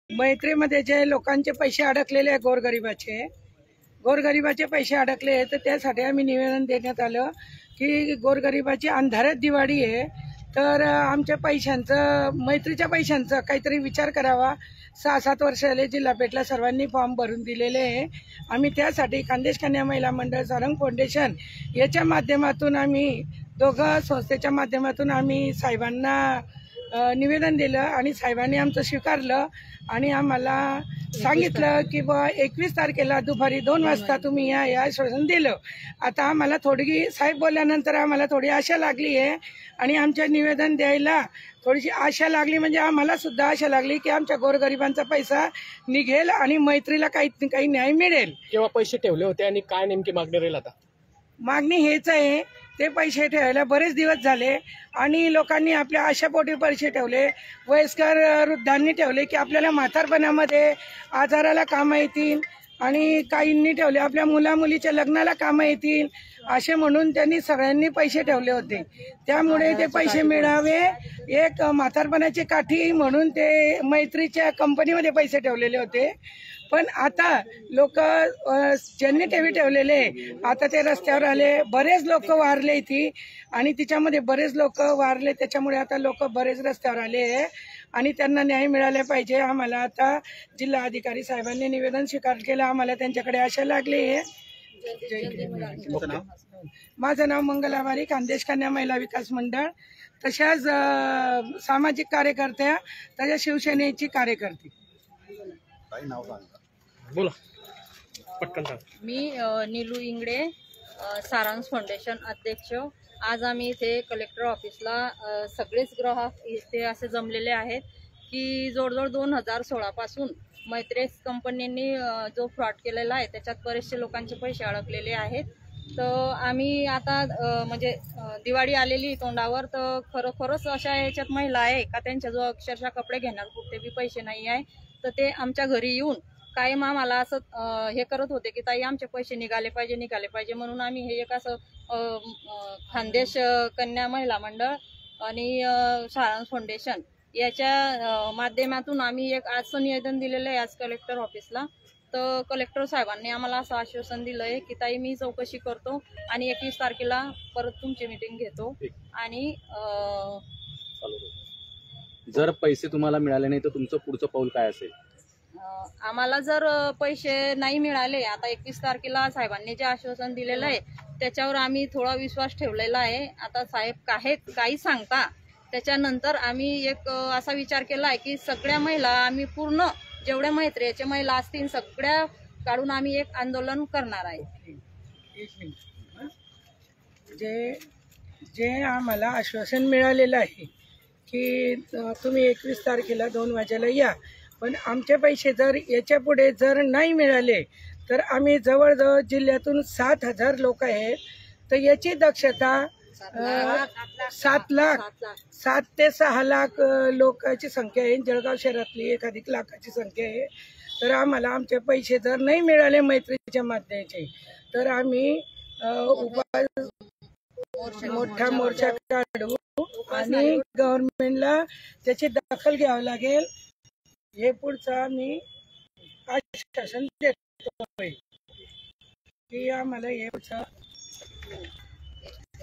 मैत्रेयाज कंपनीतील ठेवी तात्काळ परत मिळवून द्या; खान्देश कन्या व महिला विकास मंडळाची मागणी
जळगाव-संदीप होले । मैत्रेयाज सर्व्हिसेस कंपनीत अनेक ठेवीदारांनी ठेवी ठेवलेल्या होत्या. त्या ठेवी तातडीने मिळाव्यात या मागणीसाठी खान्देश कन्या व महिला विकास मंडळाच्या वतीने जिल्हाधिकारी अमन मित्तल यांना निवेदन देण्यात आले. याप्रसंगी संघटनेच्या संस्थापक अध्यक्षा मंगला बारी, प्रमिला बारी, भुपेंद्र शिनकर, वसंत बारी, राजेंद्र बारी, संजय बारी, विलास खडके, शोभा बारी, निर्मला बारी, सुरेखा बारी, धनराज कोळी, कासाबाई बारी, वंदना बारी, छाया बारी, सुषमा बारी, रेशम बडगुजर, निलू इंगळे, शारदा इंगळे यांच्यासह आदी उपस्थित होते.
See less